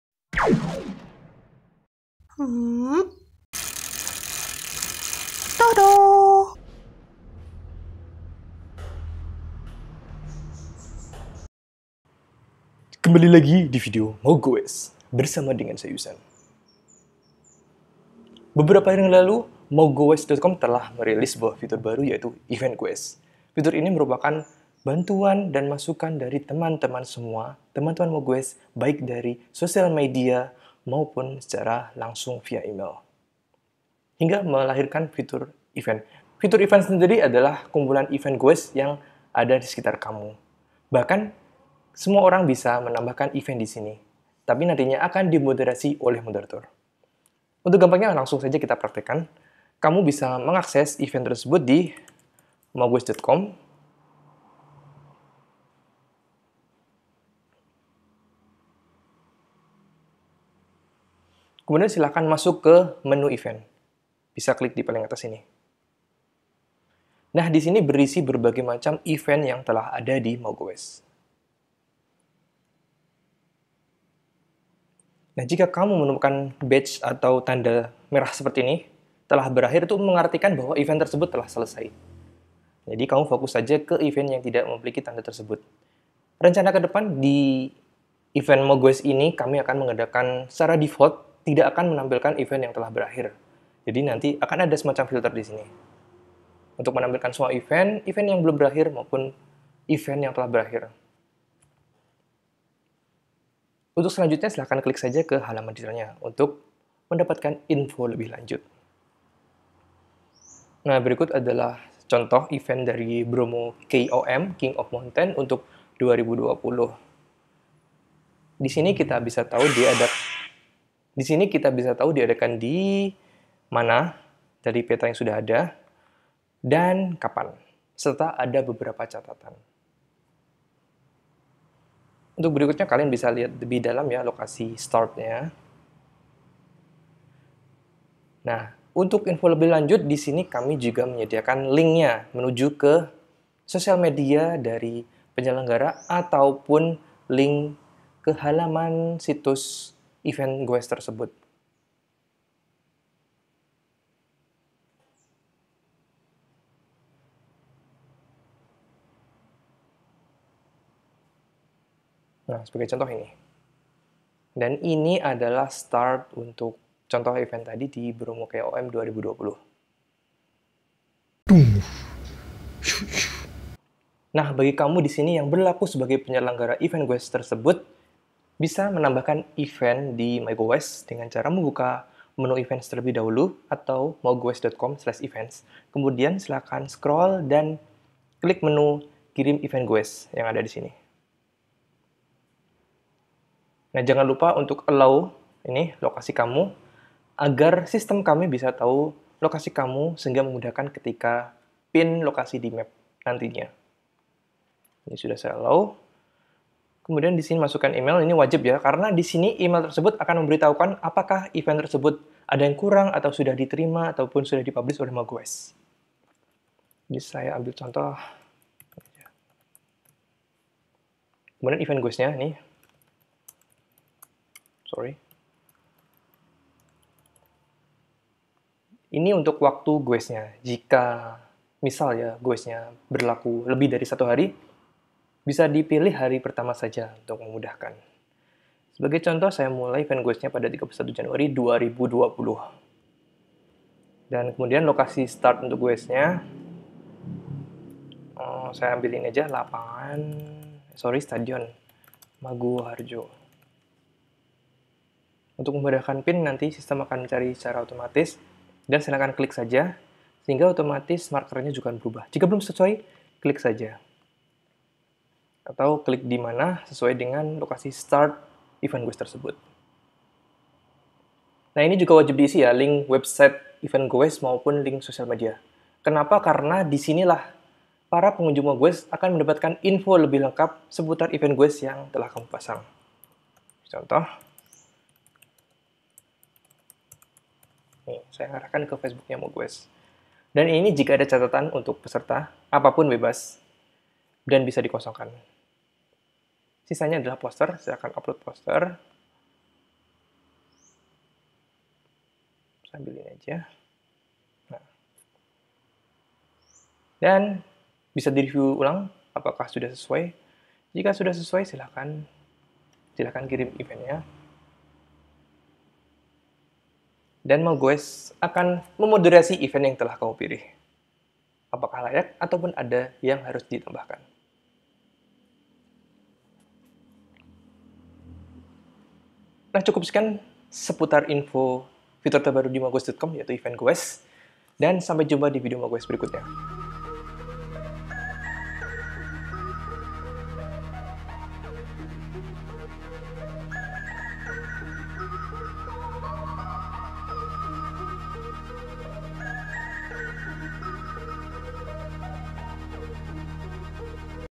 Mengenal Fitur Events di MauGowes.com | Progress Report | Mau Gowes
Fitur events di maugowes.com sangat cocok untuk kamu yang ingin gowes bareng, mengikuti event gowes atau bahkan memasang event gowes di maugowes.com . Semua orang bisa memasang event gowes di MauGowes.com, untuk kemudian akan di moderasi oleh moderator Mau Gowes. Silahkan kunjungi di

MauGowes powered by Yussan Media Group